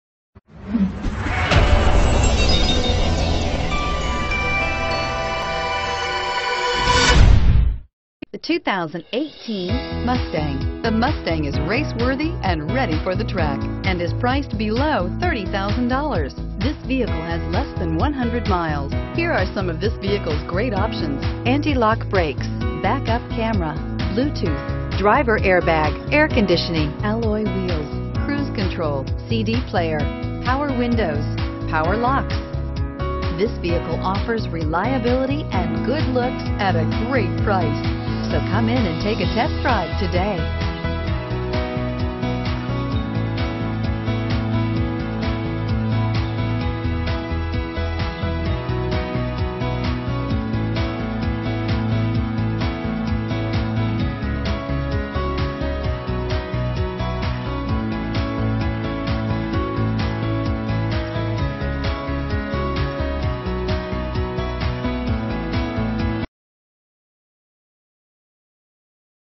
2018 Ford Mustang RIVERSIDE,TEMECULA,SAN BERNARDINO,MORENO VALLEY,FONTANA 81784
2018 Ford Mustang EcoBoost Fastback

Fritts Ford
8000 Auto Drive

FRITTS FORD, PROUDLY SERVING RIVERSIDE, TEMECULA, SAN BERNARDINO, MORENO VALLEY, FONTANA, AND SURROUNDING CALIFORNIA LOCATIONS. THIS OXFORD WHITE 2018 Ford MUSTANG IS EQUIPPED WITH A 2.3L EcoBoost, AUTOMATIC TRANSMISSION, AND RECEIVES AN ESTIMATED 21 City/31 Hwy MPG. 2018 Ford MUSTANG EcoBoost Fastback Stock #: 81784 | VIN: 1FA6P8TH1J5180962 | 2018 Ford Mustang EcoBoost Fastback Fritts Ford